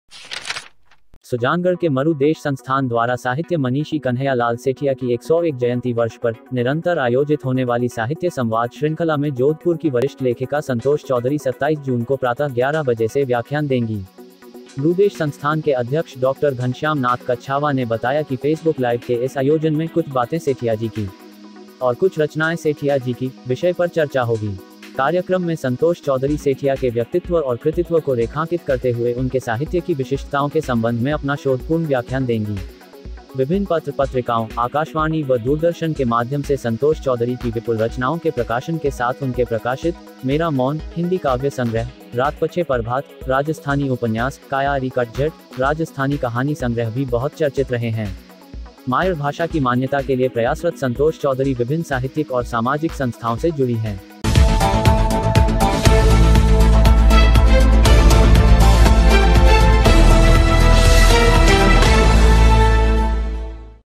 साहित्य मनीषी कन्हैयालाल सेठिया साहित्य संवाद 27 जून को II INTERVIEWER INDIA II
फेसबुक लाइव कार्यक्रम में जोधपुर की वरिष्ठ लेखिका संतोष चौधरी करेंगी व्याख्यान।

सुजानगढ़। स्थानीय मरूदेश संस्थान द्वारा साहित्य मनीषी कन्हैया लाल सेठिया की एक सौ एक जयन्ती वर्ष पर निरन्तर आयोजित होने वाली साहित्य संवाद श्रृंखला में जोधपुर की वरिष्ठ लेखिका संतोष चौधरी 27 जून को प्रातः ग्यारह बजे व्याख्यान देंगी। मरूदेश संस्थान के अध्यक्ष डॉ. घनश्याम नाथ कच्छावा ने बताया कि फेसबुक लाइव इस आयोजन में "कुछ बातें सेठिया जी की , कुछ रचनाएं सेठिया जी की " विषय पर चर्चा होगी। कार्यक्रम में संतोष चौधरी सेठिया के व्यक्तित्त्व और कृतित्व को रेखांकित करते हुए उनके साहित्य की विशिष्टताओं के सम्बन्ध में अपना शोधपूर्ण व्याख्यान देंगी। विभिन्न पत्र -पत्रिकाओं,आकाशवाणी व दूरदर्शन के माध्यम से संतोष चौधरी की विपुल रचनाओं के प्रकाशन के साथ उनके प्रकाशित 'मेरा मौन' हिंदी काव्य संग्रह,'रात पछै परभात' राजस्थानी उपन्यास , 'काया री कळझळ' राजस्थानी कहानी संग्रह भी बहुत चर्चित रहे हैं। मायड़ भाषा की मान्यता के लिए प्रयासरत संतोष चौधरी विभिन्न साहित्यिक और सामाजिक संस्थाओं से जुड़ी हैं।